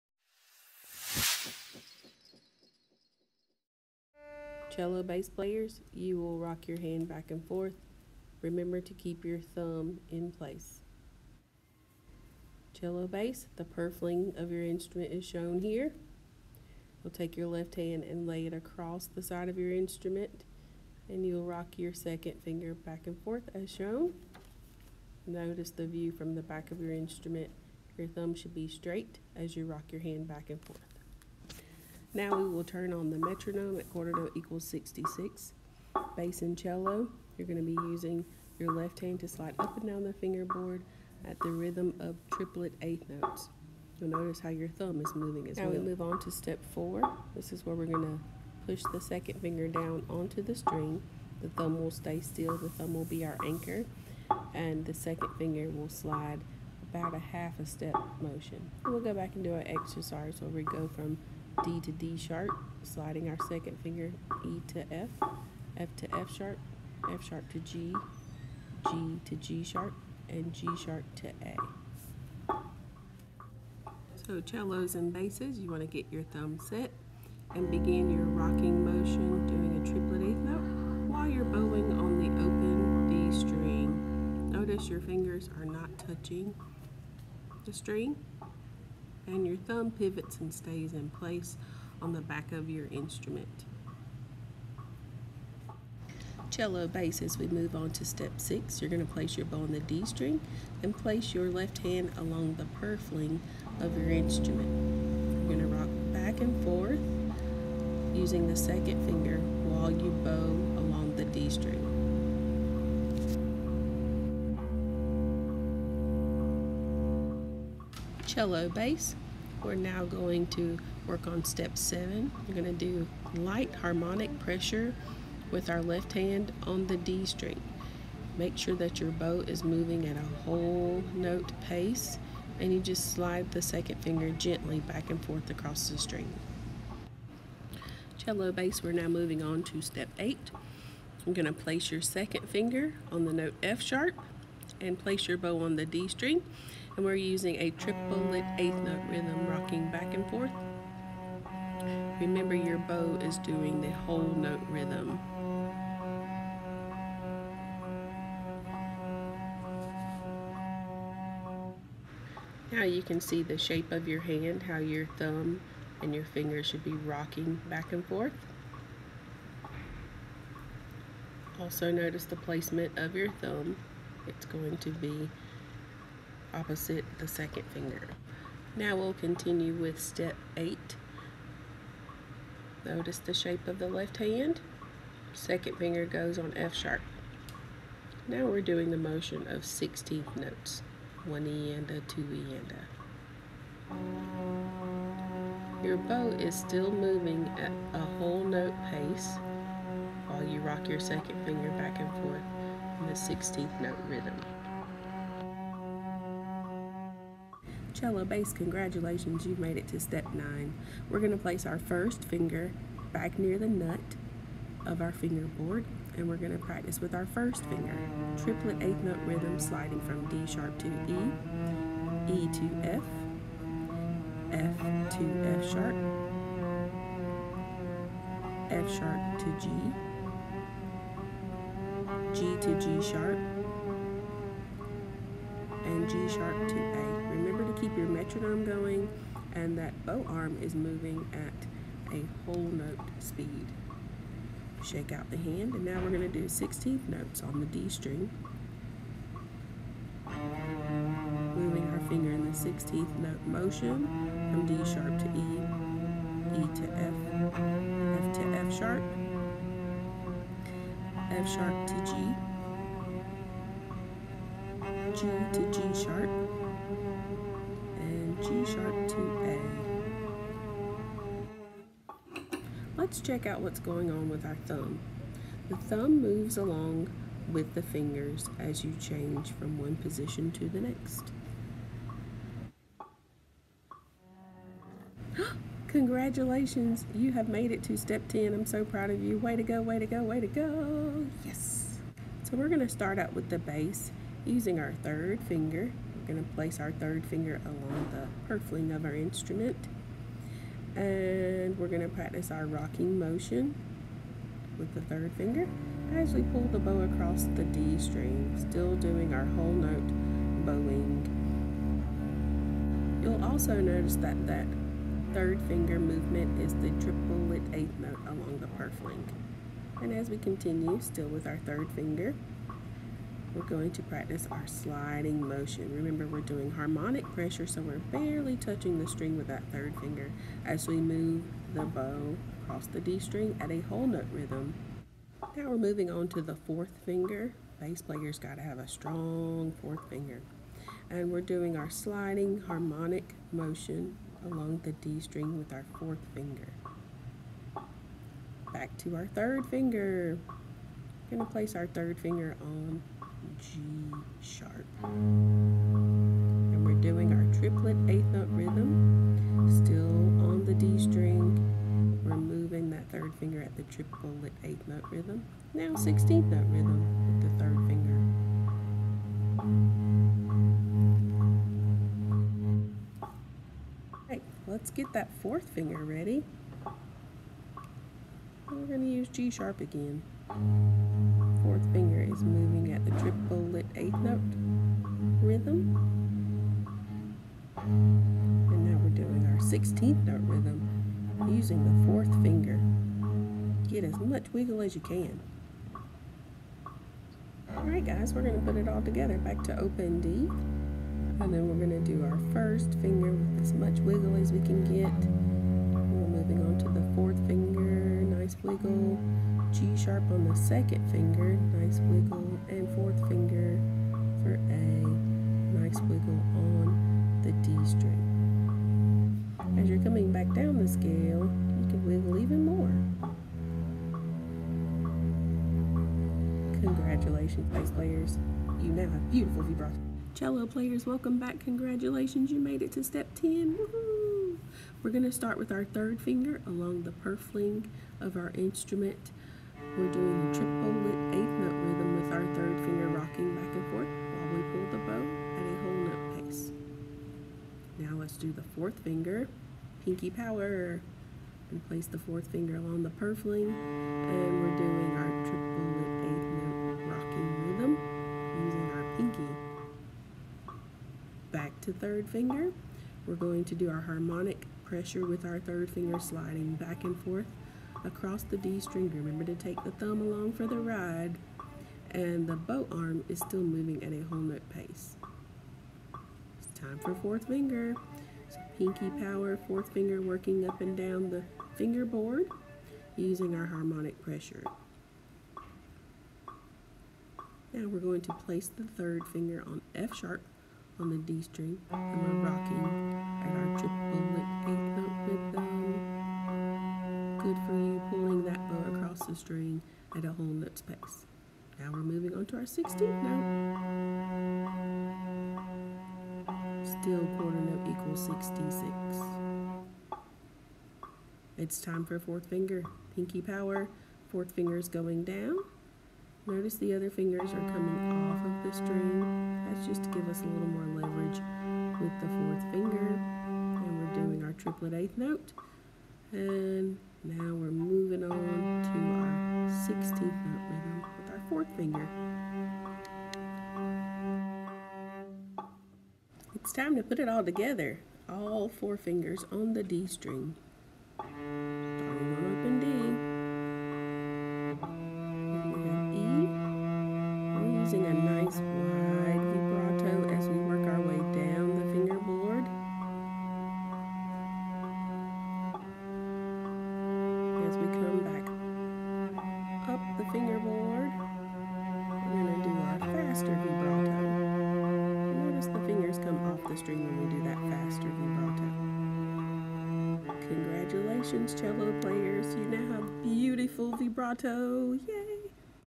Beautiful vibrato in 10 steps for cellos and basses
Do the following steps, one per week, to perfect your vibrato. By week 10, you’ll be able to vibrate all four fingers on the D string. Be sure to spend 1 to 5 minutes per day working on your vibrato exercises.

Here's a link to use in your classroom with Google Slides.